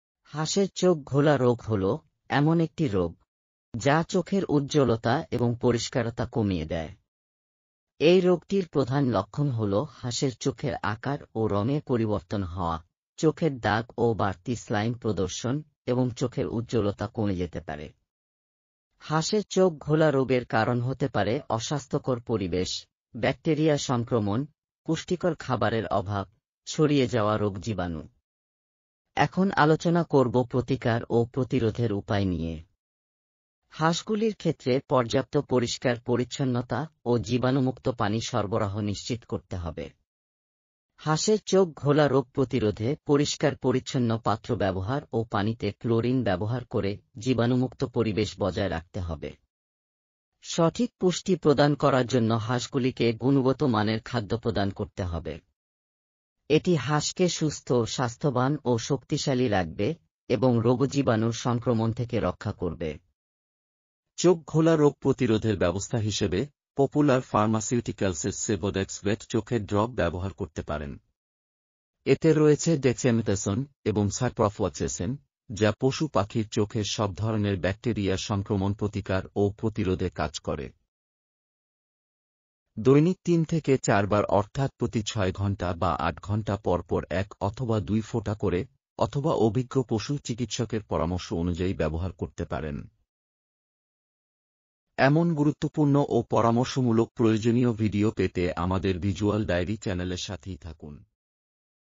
হাঁসের চোখ ঘোলা রোগ | Eye Disease | Eye Drop | Duck | হাঁসের ড্রপ
হাঁসের চোখ ঘোলা রোগ - Duck's Eye Disease
হাঁসের চোখ ঘোলা রোগ হলো এমন একটি রোগ যা চোখের উজ্জ্বলতা এবং পরিস্কারতা কমিয়ে দেয়। এই রোগটির প্রধান লক্ষণ হলো হাসের চোখের আকার ও রঙে পরিবর্তন হওয়া, চোখের দাগ ও বাড়তি স্লাইম প্রদর্শন এবং চোখের উজ্জ্বলতা কমে যেতে পারে।
হাঁসের চোখ ঘোলা রোগের কারণ হতে পারে অস্বাস্থ্যকর পরিবেশ, ব্যাকটেরিয়ার সংক্রমণ, পুষ্টিকর খাবারে অভাব, ছড়িয়ে যাওয়া রোগজীবাণু।
এখন আলোচনা করবো প্রতিকার ও প্রতিরোধের উপায় নিয়ে।
হাঁসগুলির ক্ষেত্রে পর্যাপ্ত পরিষ্কার পরিচ্ছন্নতা ও জীবাণুমুক্ত পানি সরবরাহ নিশ্চিত করতে হবে। হাঁসের চোখ ঘোলা রোগ প্রতিরোধে পরিষ্কার পরিচ্ছন্ন পাত্র ব্যবহার ও পানিতে ক্লোরিন ব্যবহার করে জীবাণুমুক্ত পরিবেশ বজায় রাখতে হবে।
 সঠিক পুষ্টি প্রদান করার জন্য হাঁসগুলিকে গুণগত মানের খাদ্য প্রদান করতে হবে। এটি হাঁসকে সুস্থ, স্বাস্থ্যবান ও শক্তিশালী রাখবে এবং রোগজীবাণুর সংক্রমণ থেকে রক্ষা করবে।
চোখ খোলা রোগ প্রতিরোধের ব্যবস্থা হিসেবে পপুলার ফার্মাসিউটিক্যালসের sivodex vet চোখের ড্রপ ব্যবহার করতে পারেন। এতে রয়েছে Dexamethasone এবং Ciprofloxacin, যা পশু পাখির চোখের সব ধরনের ব্যাকটেরিয়ার সংক্রমণ প্রতিকার ও প্রতিরোধে কাজ করে।
দৈনিক তিন থেকে চারবার অর্থাৎ প্রতি ছয় ঘণ্টা বা আট ঘণ্টা পরপর এক অথবা দুই ফোঁটা করে অথবা অভিজ্ঞ পশু চিকিৎসকের পরামর্শ অনুযায়ী ব্যবহার করতে পারেন।
এমন গুরুত্বপূর্ণ ও পরামর্শমূলক প্রয়োজনীয় ভিডিও পেতে আমাদের ভিজুয়াল ডায়েরি চ্যানেলের সাথেই থাকুন। ধন্যবাদ।